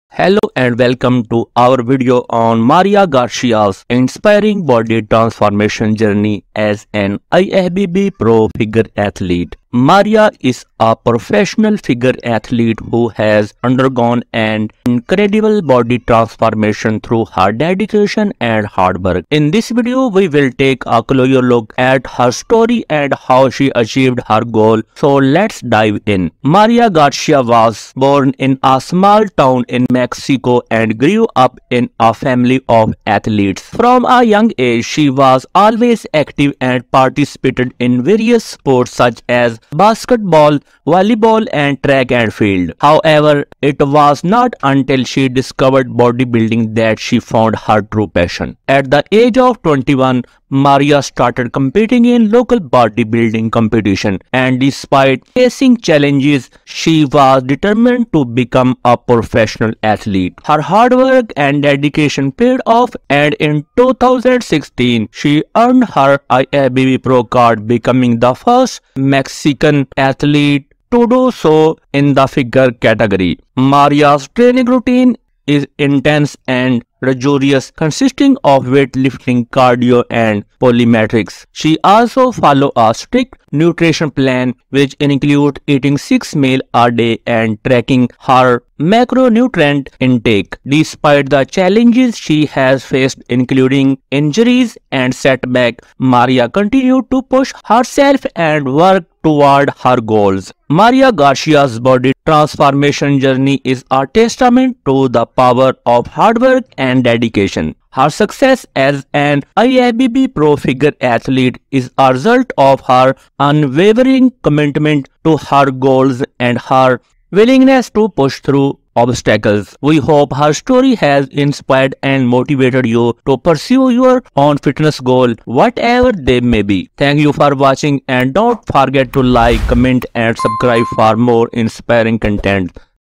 Watch Maria's Unbelievable Body Transformation! | fbb Warriors
FBB Maria Garcia's Inspiring Body Transformation Journey || fbb warriors

In this video, we'll take a closer look at the incredible body transformation journey of Maria Garcia, a professional figure athlete in the International Federation of Bodybuilding and Fitness (IFBB). We'll explore her background, how she got into bodybuilding, and the challenges she faced on her path to becoming an IFBB PRO. We'll also dive into her training and nutrition routines and how she overcame obstacles to reach her goals. Maria's story is one of perseverance, dedication, and hard work, and we hope it will inspire and motivate you to achieve your fitness goals as well.